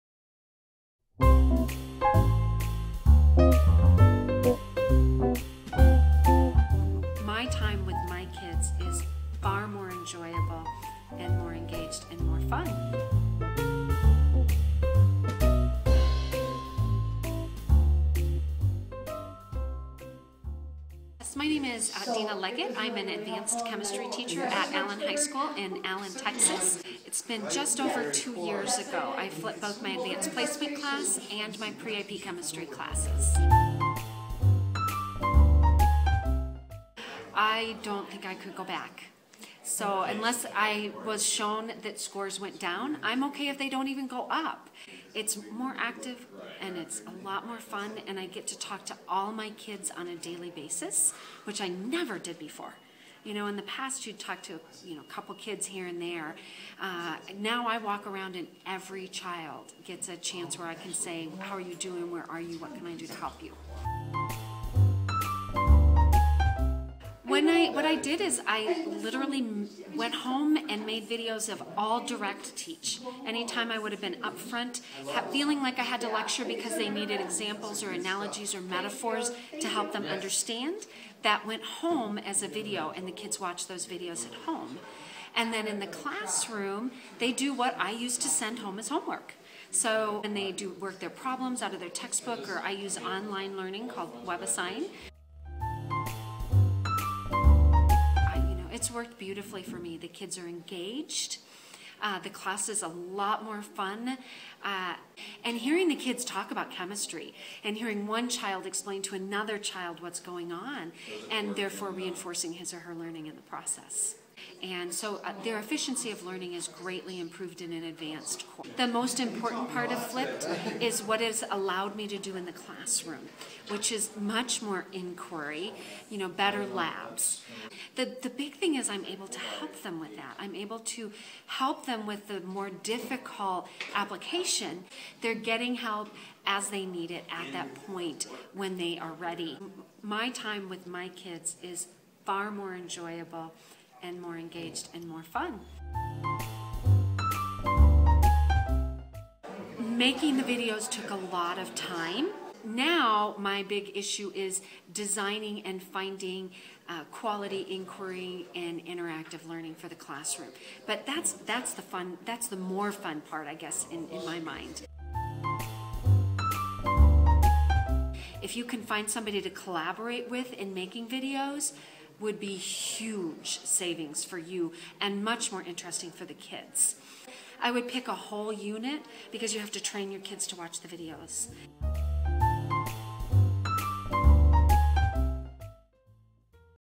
Classe inversée: chimie en lycée (D. Leggett)
Dena Leggett, enseignante en chimie en lycée, répond a nos questions sur ses classes inversées.